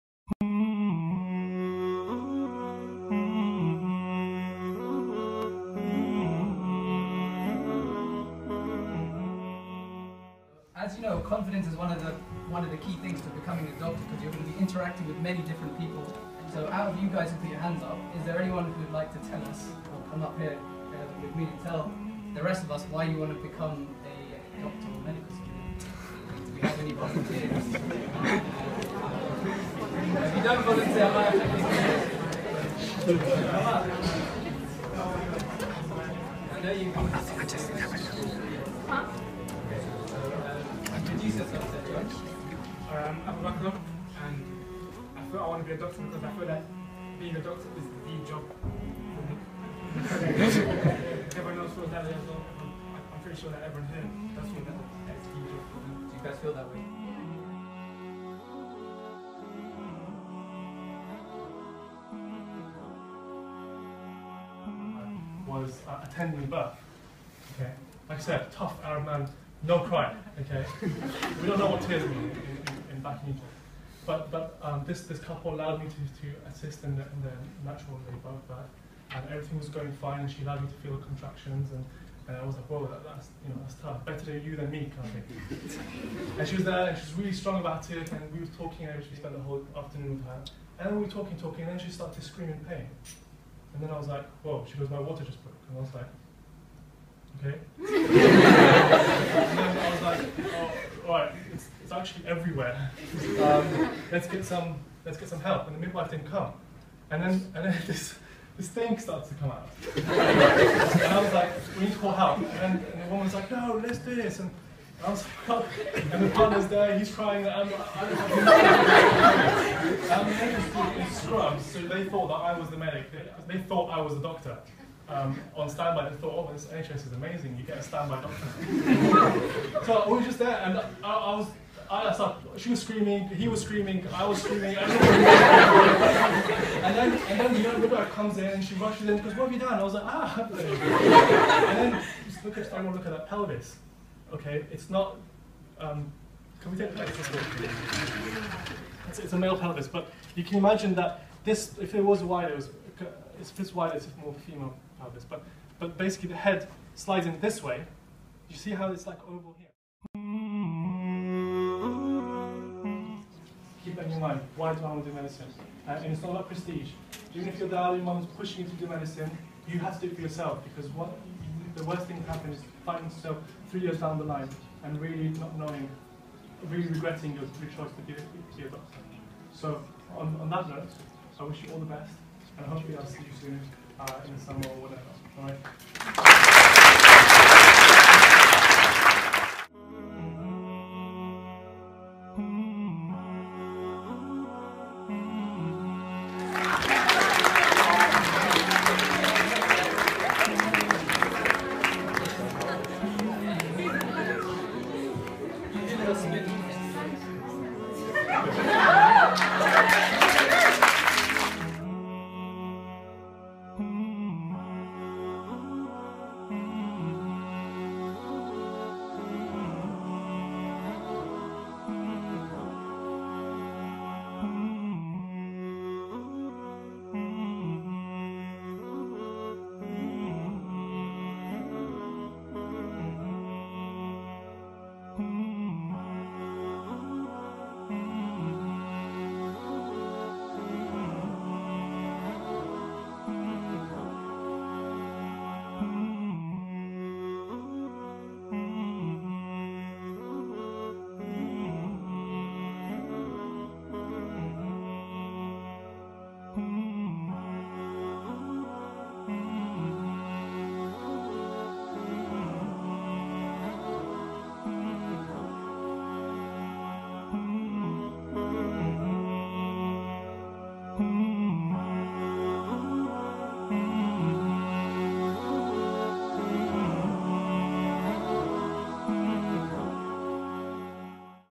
KCL Mission medicine 2015
KCL Mission Medicine is a student led society based at King's College London. Our aim is to give sixth form pupils from underrepresented backgrounds a fighting chance to study medicine at university.

These are some of our highlights from the year!